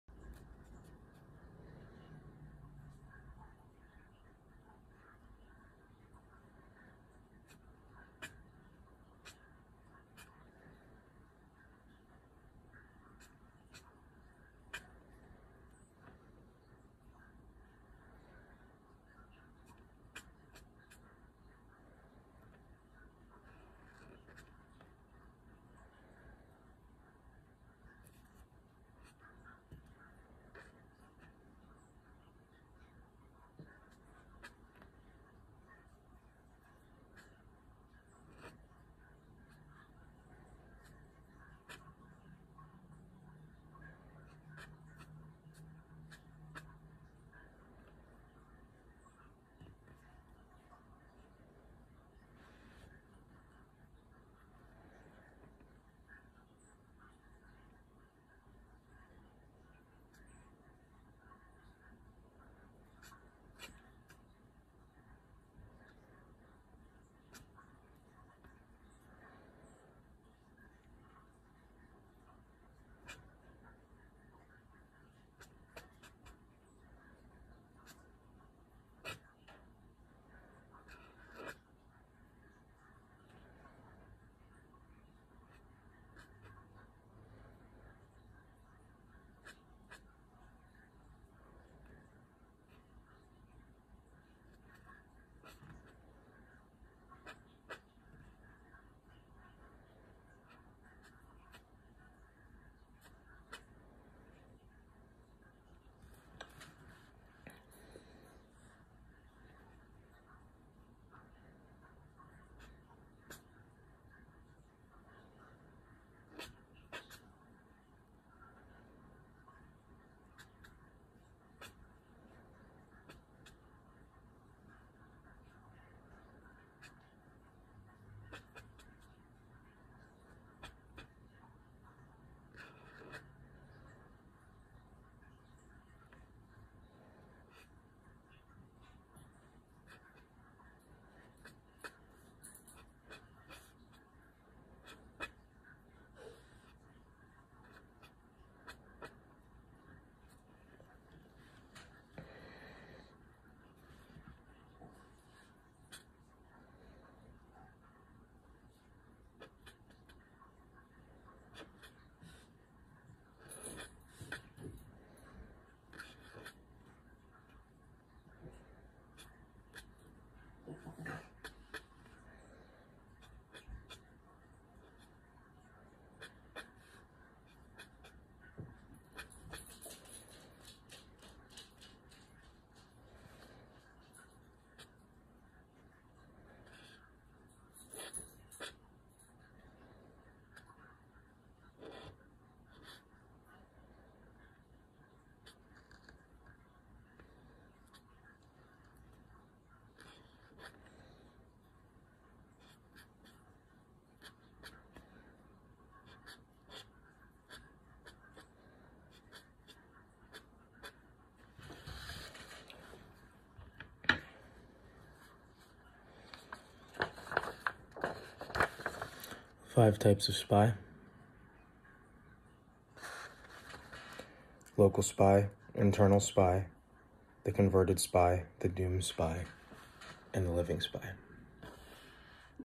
The Gokan Ideograms
Writing the ideograms for the Gokan (5 Types of Spies)